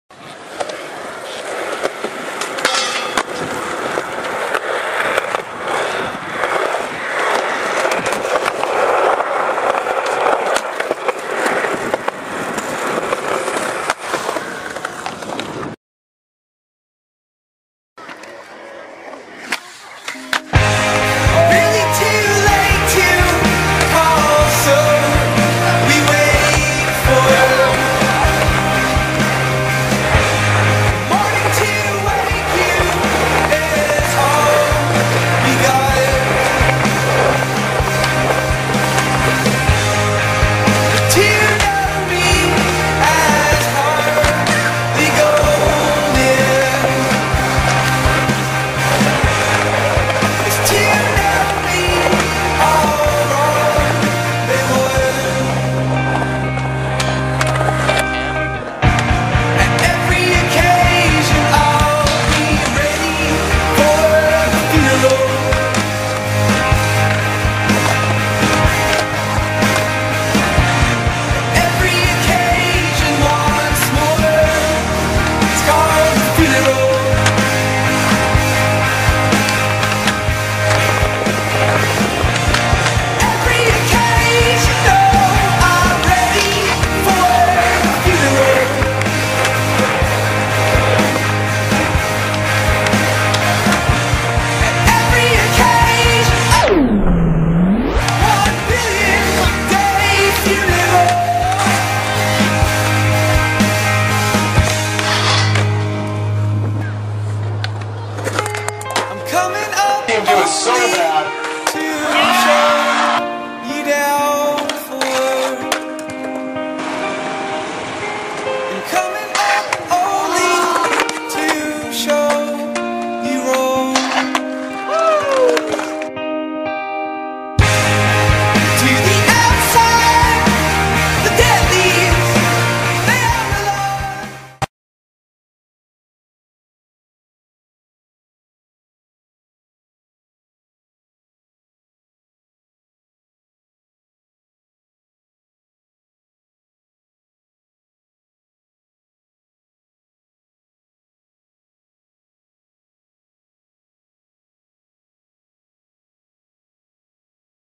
Cameron Lynch- Sponsor me/ summer 09
cameron lynch's sponsor tape/ summer 09. filmed and edited by tim mitchell. NEW TAPE IN THE PROCESS.